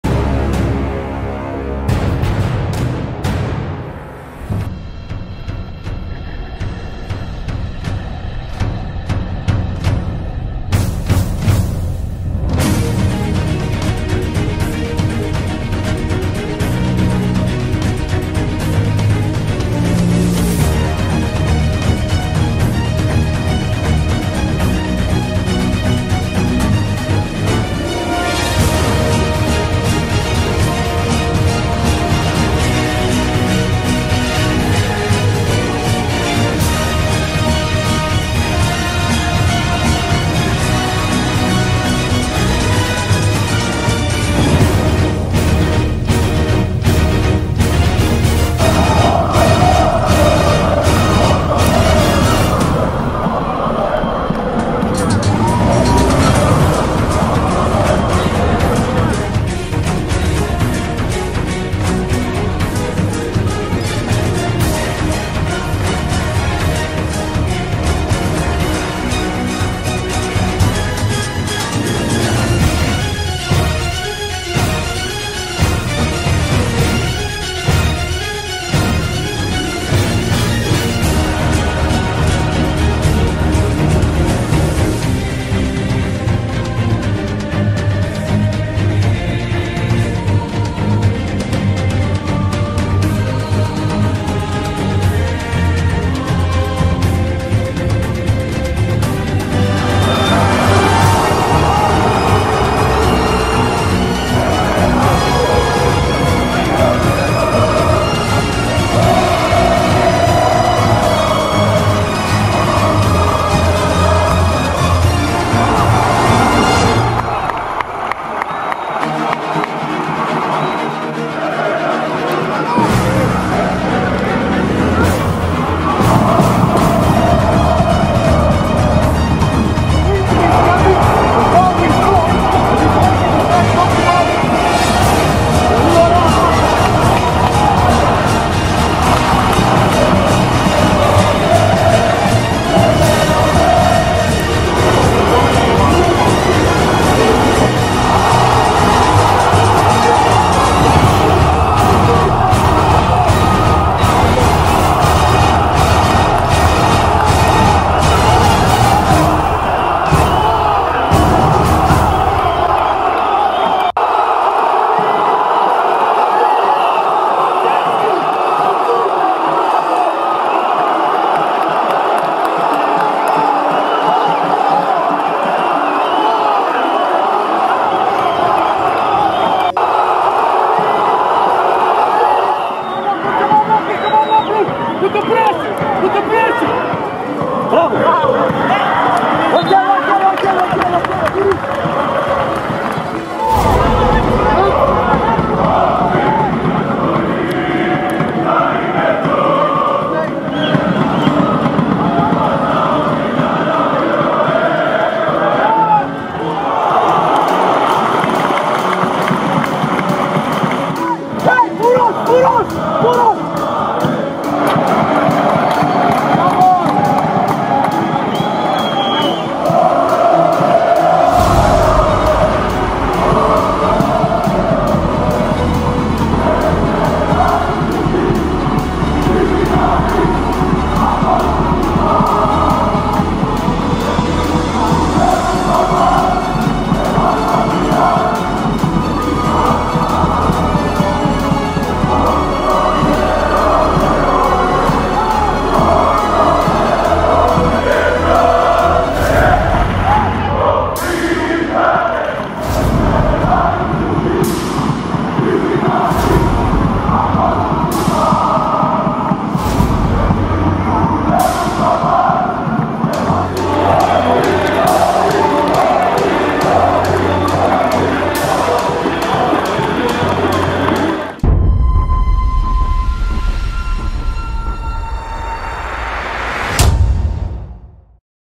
Επιβράβευση το δυνατό χειροκρότημα στο φινάλε (6/12/21)
Δείτε όλα όσα κατέγραψε η παρακάμερα του ΟΦΗ TV από την αναμέτρηση της ομάδας μας με τον Ολυμπιακό.